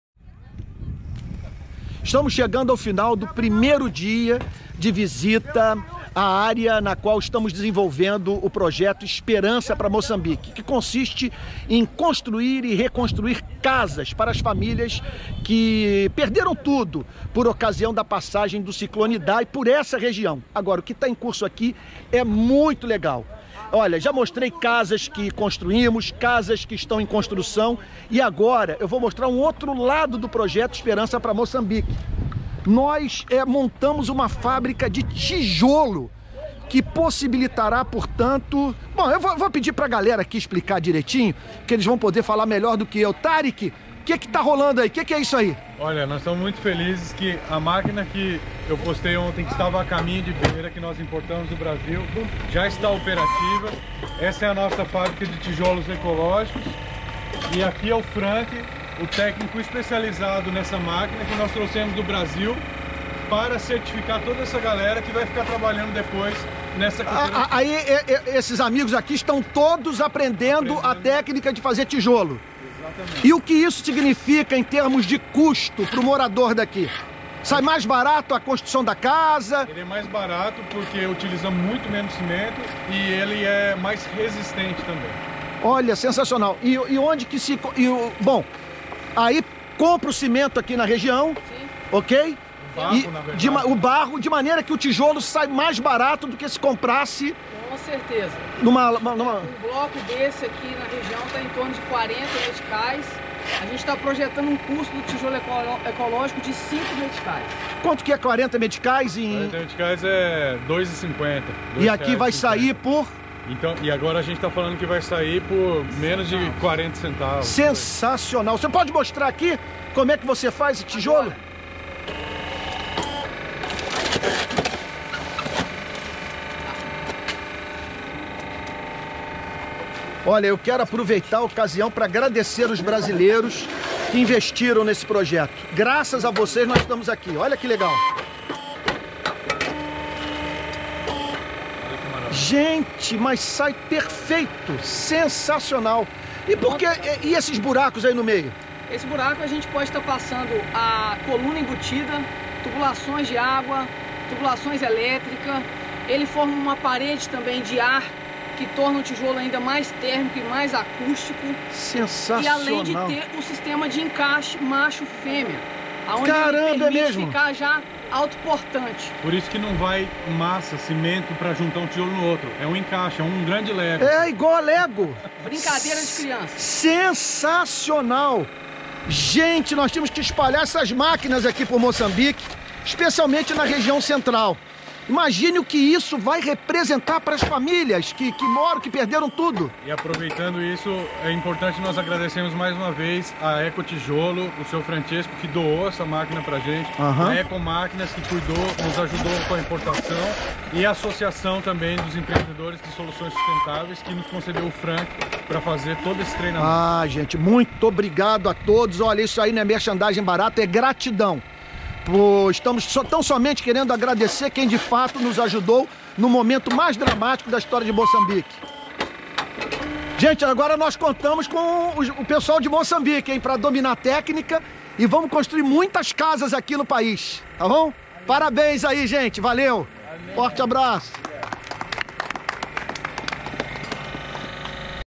FÁBRICA DE TIJOLO Direto de Moçambique
O Rio de Paz deu início a uma fábrica de tijolo em Moçambique. Enviamos um técnico para treinar os moçambicanos a fim de que eles próprios possam construir o material que será usado na edificação das casas para os desabrigados.

Tudo isso faz parte do projeto “Esperança para Moçambique”.

Sua ajuda fará diferença para pessoas que vivem abaixo da linha da pobreza